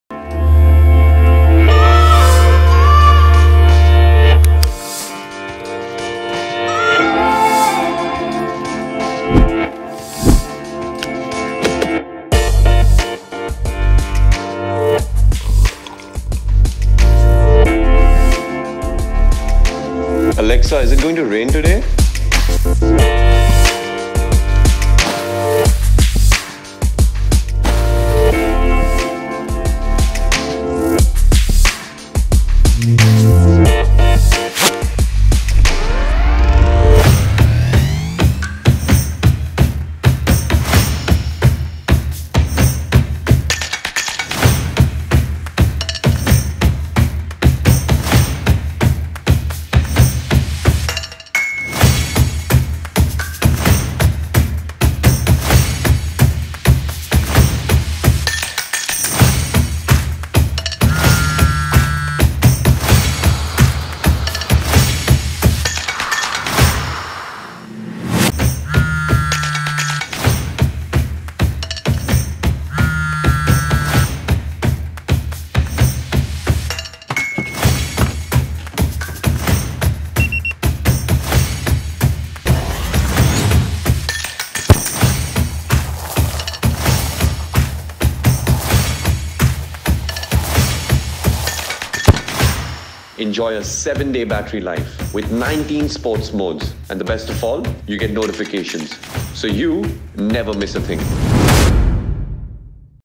OriGen Armour Pro Smartwatch
Presenting Armour Pro, OriGen's state-of-the-art smartwatch!
Armour Pro, is a watch that will revolutionize your life.
Track your activity, talk to Alexa, listen to music, check your SpO2, stress levels and much more. The possibilities are endless!